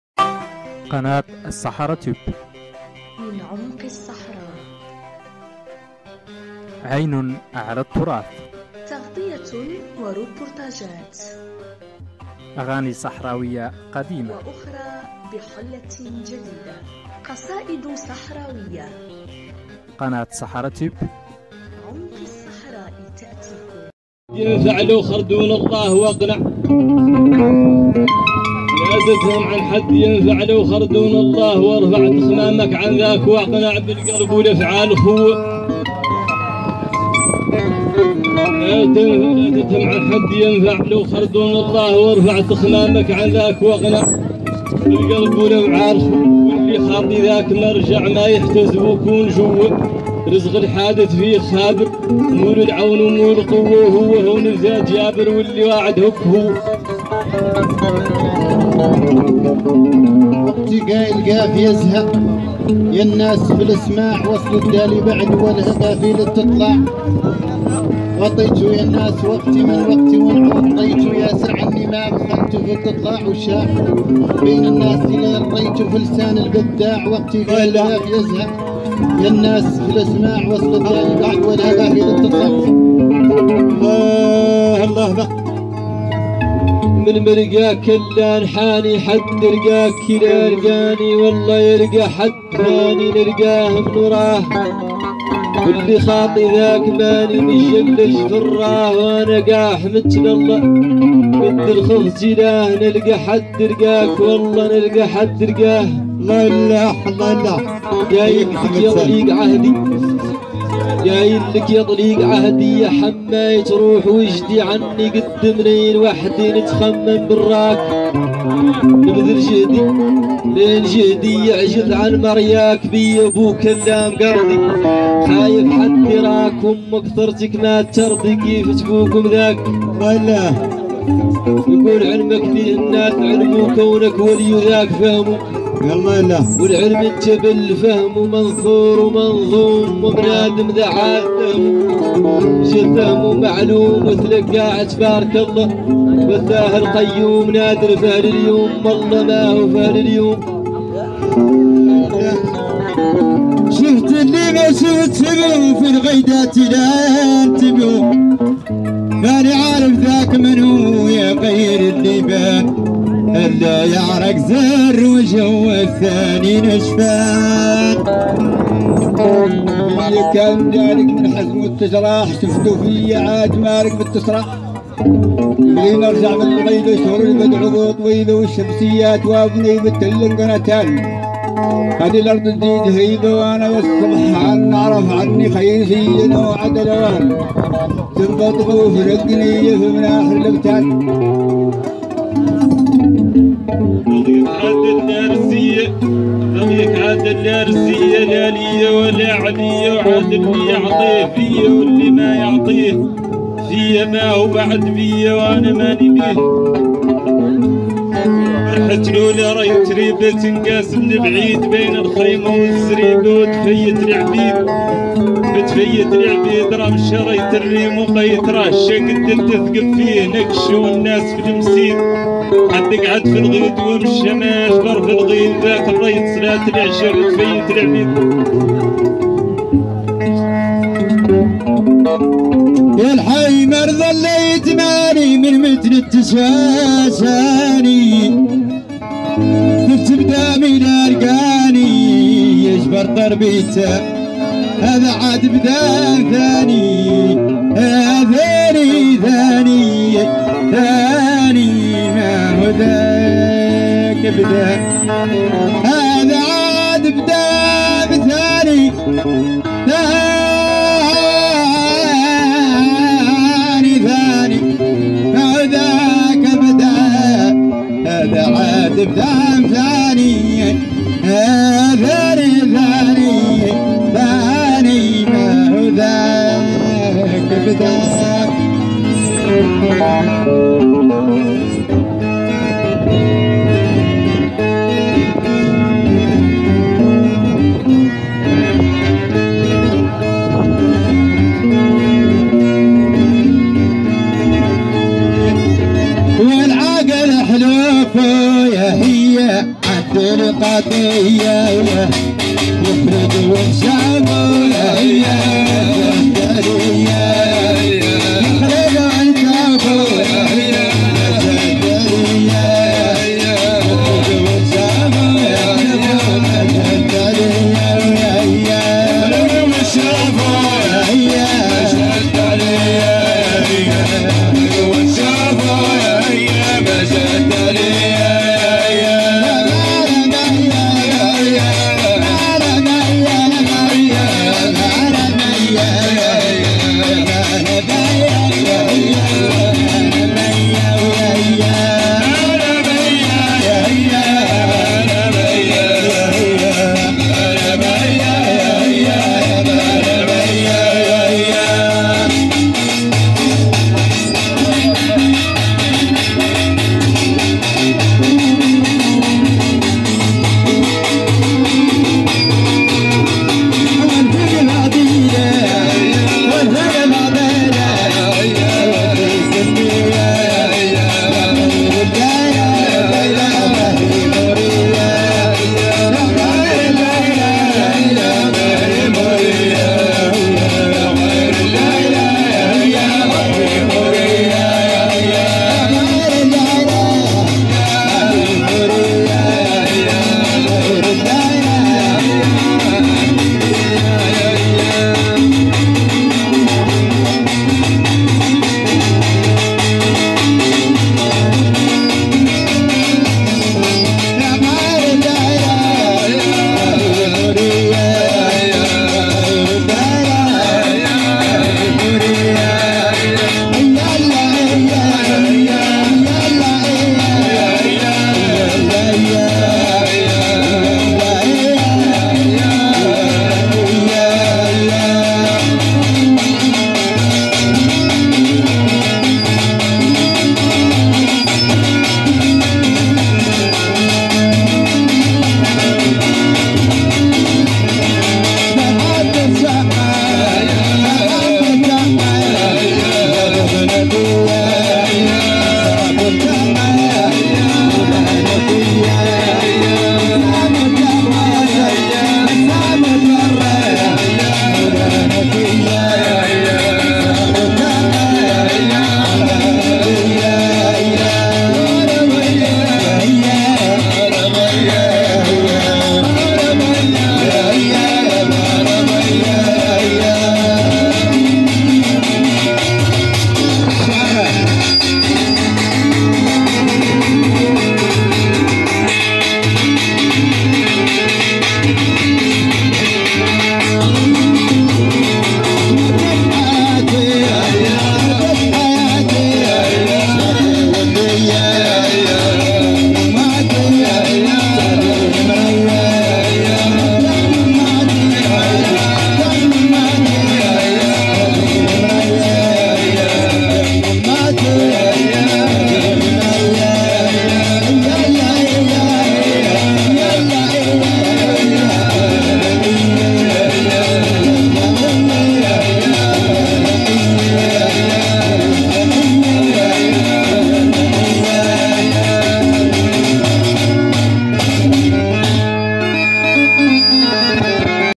أجمل سهرة شعرية صحراوية من الشعر الحساني الطلع والكيفان
سهرة فنية شعرية حسانية من إنتاج وتصوير قناتنا
هذا الفيديو قمنا بتصويره من سهرة فنية وامسية شعرية خلال مهرجان حاسي الكاح 2018
سهرة شعرية صحراوية رائعة من مهرجان حاسي الكاح
احياها 5 شعراء هم الشاعر احمد الغيط . الشاعر عالي احميميد . الشاعر محمد بوحمرة . الشاعر لحبيب الجميعي . والشاعر عبد الله المرواني.
بتنشيط من احدى المجموعات الصحراوية بقيادة الفنان العبادلة اباه